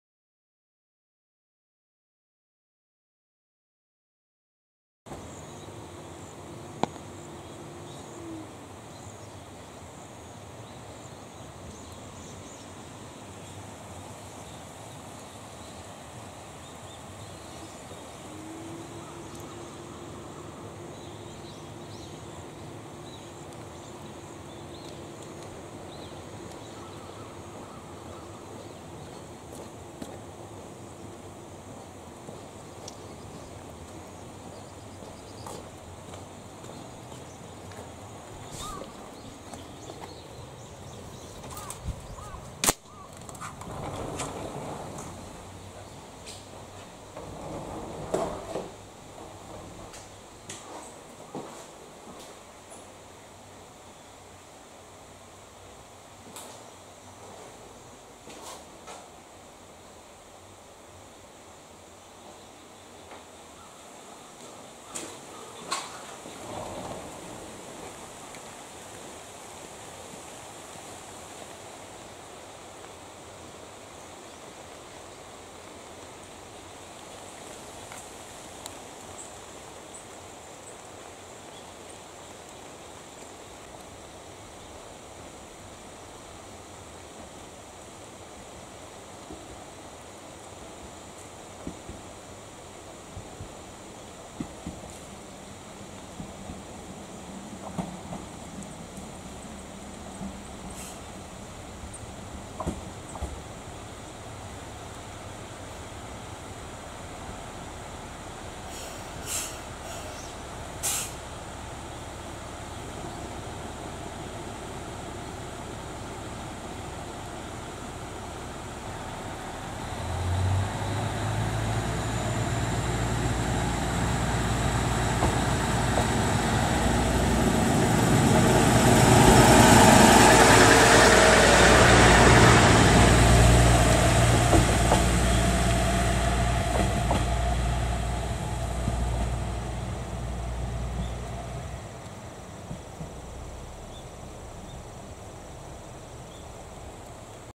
【山深い駅】飯山線 越後鹿渡駅の構内風景と発着するキハ110（2024.9.22撮影）
飯山線 越後鹿渡駅の構内風景と発着するキハ110を撮影しました。
国道353号に面した駅ですが、非常に山深い感じのする駅でした。